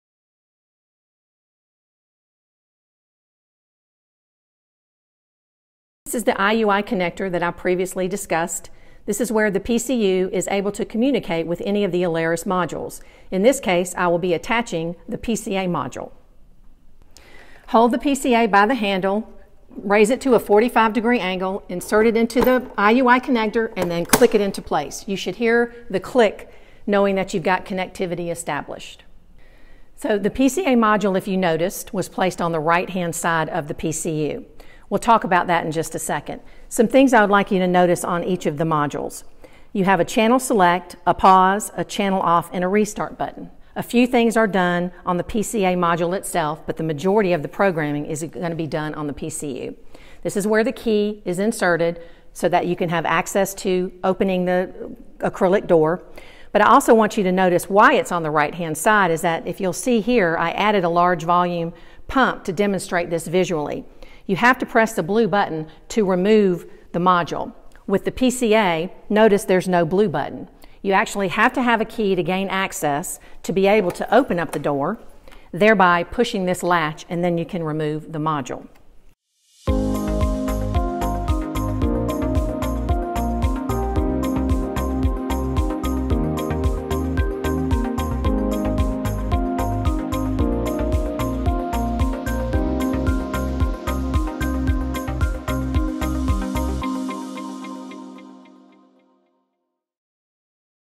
Attaching the Alaris 8120 Module
One of the many benefits of the Alaris system is it’s modular capability, up to 4 modules can be connected to a single 8015 PCU. In this video, we show you how to connect the Alaris 8120 to the PCU in preparation for programming an infusion.